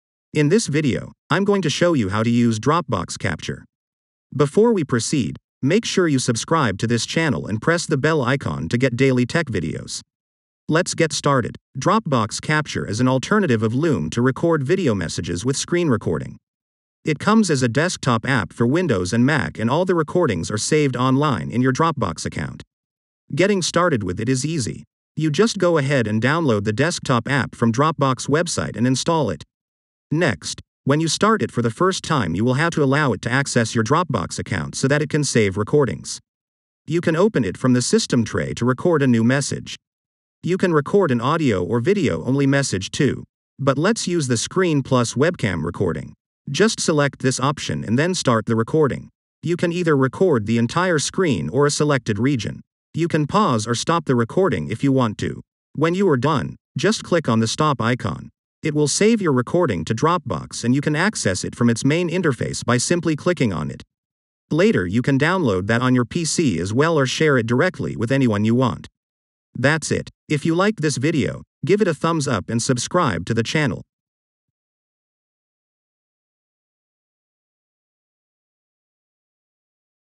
How to Use Dropbox Capture: Loom Alternative to Record Video Messages with Screen Recording
Dropbox Capture is an alternative of Loom to record video messages with screen recording. It comes as a desktop app for Windows and Mac and all the recordings are saved online in your Dropbox account.

Getting started with it is easy. You just go ahead and download the desktop app from Dropbox website and install it.

 Next, when you start it for the first time you will have to allow it to access your Dropbox account so that it can save recordings.

You can open it from the system tray to record a new message. You can record an audio or video only message too. but let's use the screen plus Webcam recording. Just select this option and then start the recording. You can either record the entire screen or a selected region.

You can pause or stop the recording if you want to.

When you are done, just click on the stop icon. It will save your recording to Dropbox and you can access it from its main interface by simply clicking on it. Later you can download that on your PC as well or share it directly with anyone you want.